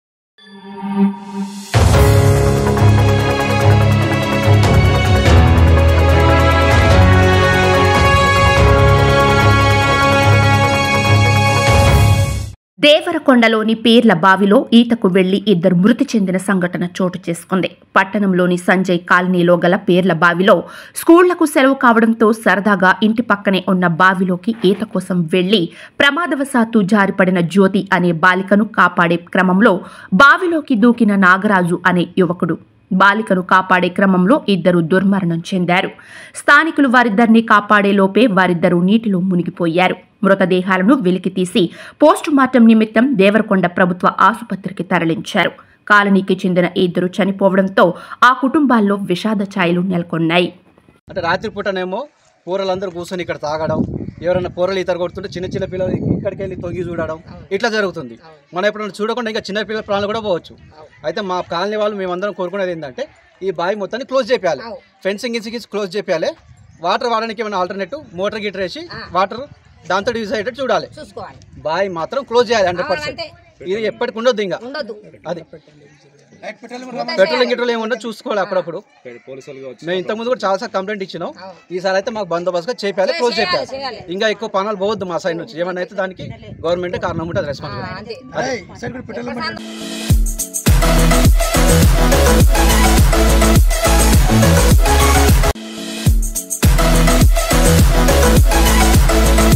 దేవరకొండలోని పీర్లబాయిలో స్కూళ్లకు సెలవుల్ కావడంతో సరదాగా ఇంటి పక్కనే ఉన్న బావిలోకి ఈత కోసం వెళ్ళి..
దేవరకొండలోని పీర్లబాయిలో స్కూళ్లకు సెలవుల్ కావడంతో సరదాగా ఇంటి పక్కనే ఉన్న బావిలోకి ఈత కోసం వెళ్ళి..|| Y9NEWS TELUGU